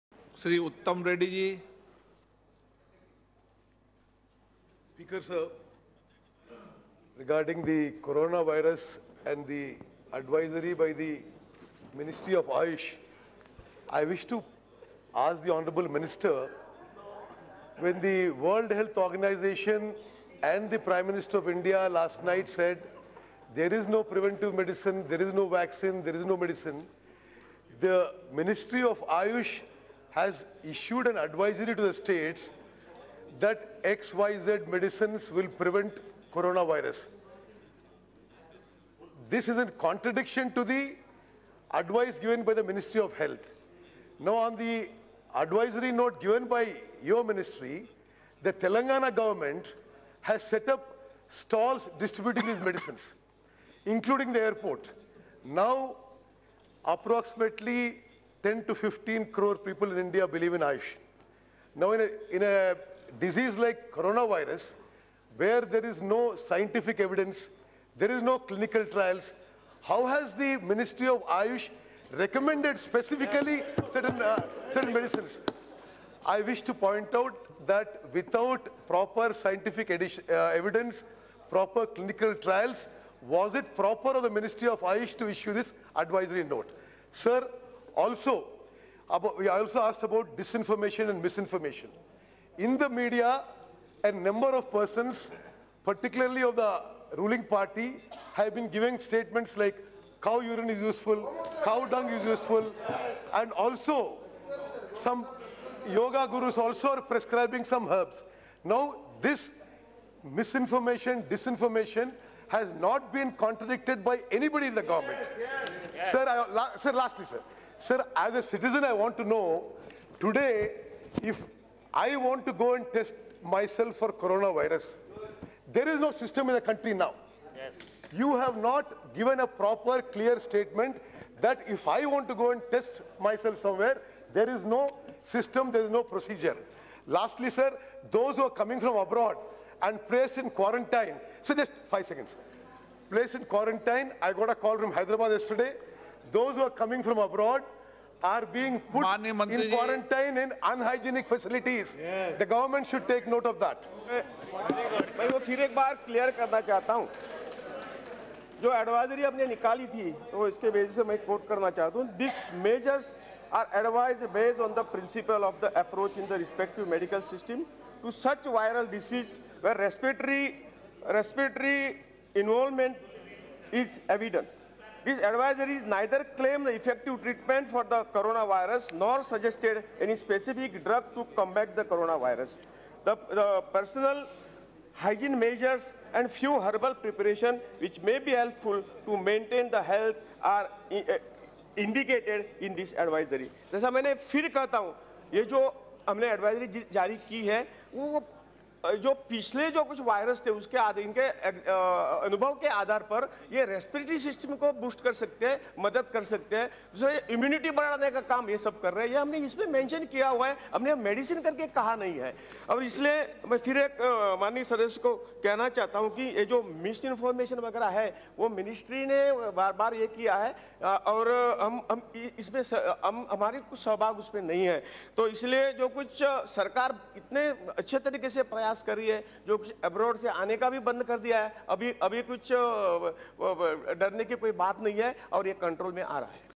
MP Uttam Kumar Reddy questions the Government on Coronavirus during Question Hour in Lok Sabha
1. ప్రపంచ ఆరోగ్య సంస్థ, ప్రధానమంత్రి గారు కరోనాకి నివారణ లేదని అన్నరు. మరి ఆయుష్ మంత్రి శాఖ కరోనాకి నివారణ అంటూ నోటీసు ఎలా జారీ చేసింది?

2. ఆయుష్ శాఖ సలహా ప్రకారం తెలంగాణ ప్రభుత్వం రాష్ట్రమంతా స్టాల్స్ పెట్టిండ్రు(విమానాశ్రయంలోనూ). ఎలాంటి శాస్త్రీయ రుజువు లేకుండా ఆయుష్ శాఖ ఇలాంటి తప్పుడు ప్రచారాలు చేయడం ఎంతవరకు సమంజసం?

MP Uttam Kumar Reddy's questions to the Government of India on COVID-19 misinformation and the Minister's response.